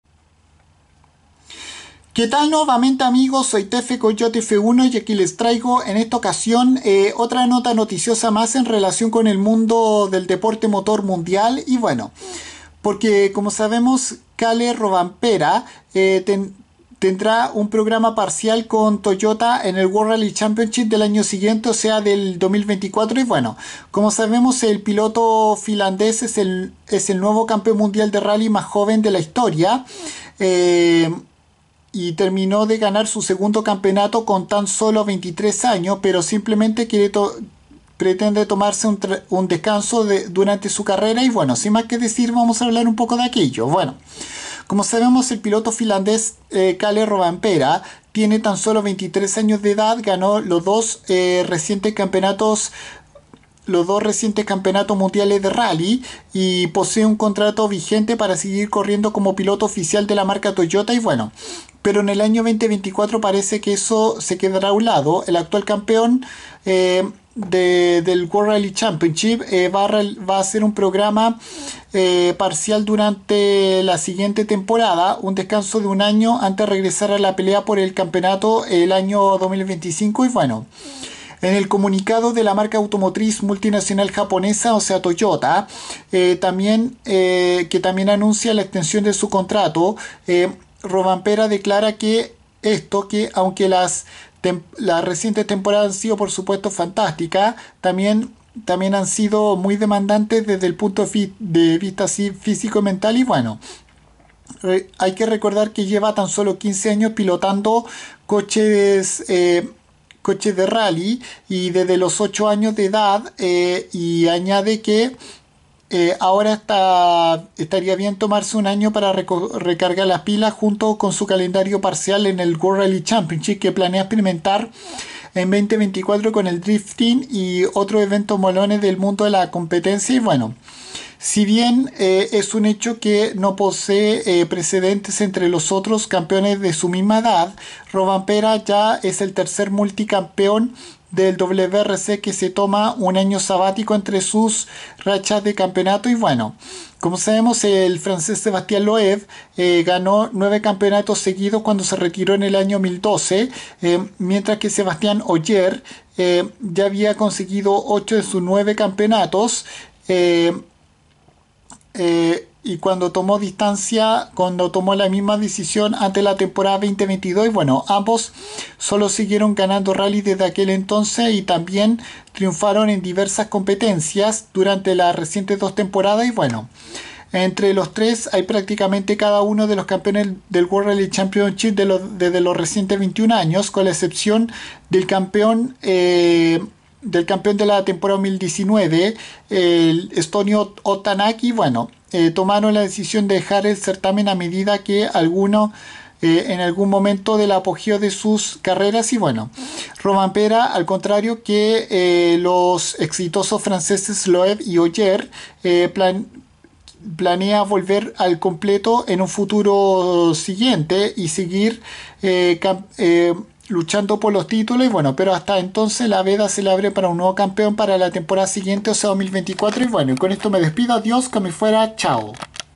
Kalle Rovanperä tendrá un programa parcial en el WRC de 2024
El joven campeón mundial finlandés del World Rally Championship ganador de la reciente Temporada 2023 con la automotriz multinacional japonesa Toyota pretende tener un descanso dentro de su carrera profesional